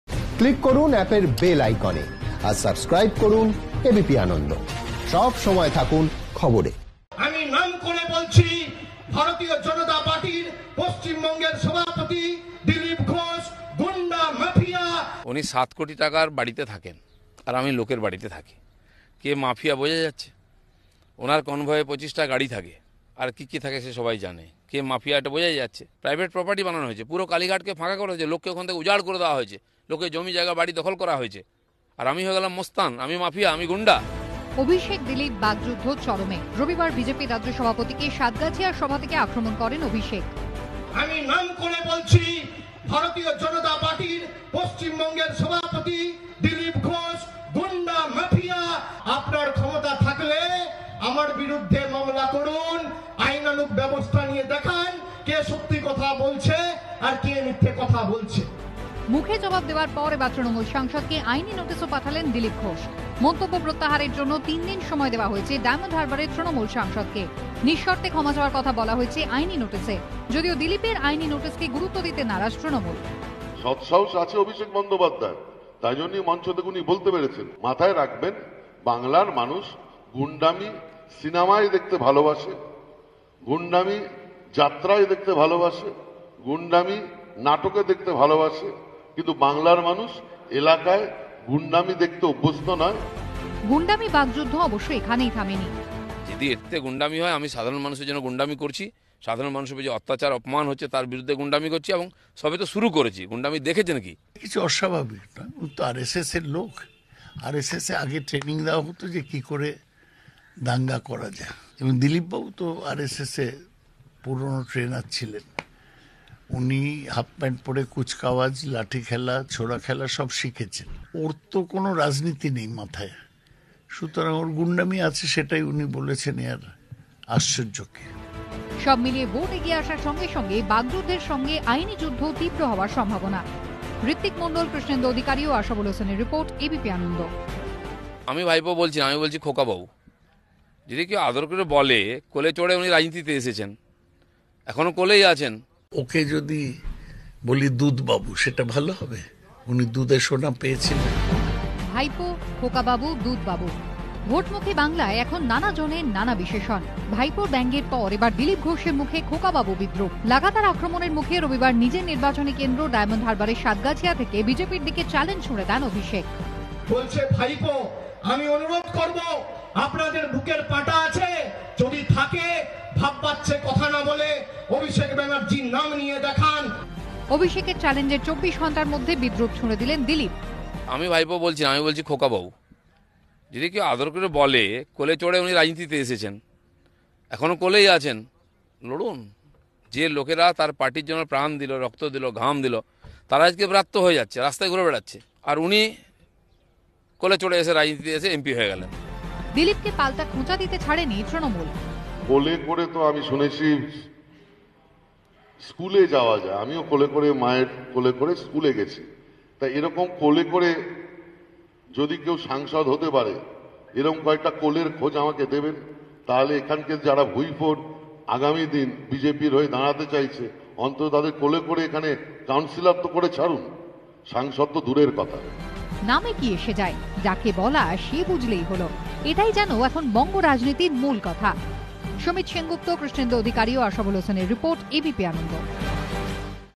West Bengal Elections 2021: 'গুণ্ডা' মন্তব্যের জন্য অভিষেককে আইনি নোটিস Dilip Ghosh এর
Abhishek Banerjee-Dilip Ghosh বাগযুদ্ধ চরমে। রবিবার BJP-র রাজ্য সভাপতি দিলীপ ঘোষকে সাতগাছিয়ার সভা থেকে আক্রমণ করেন অভিষেক বন্দ্যোপাধ্যায়। মুখে জবাব দেওয়ার পর এবার তৃণমূল সাংসদকে Legal Notice-ও পাঠালেন দিলীপ ঘোষ। মন্তব্য প্রত্যাহারের জন্য তিনদিন সময় দেওয়া হয়েছে ডায়মন্ড হারবারের তৃণমূল সাংসদকে। নিঃশর্তে ক্ষমা চাওয়ার কথা বলা হয়েছে আইনি নোটিশে। যদিও দিলীপের আইনি নোটিশকে গুরুত্ব দিতে নারাজ তৃণমূল।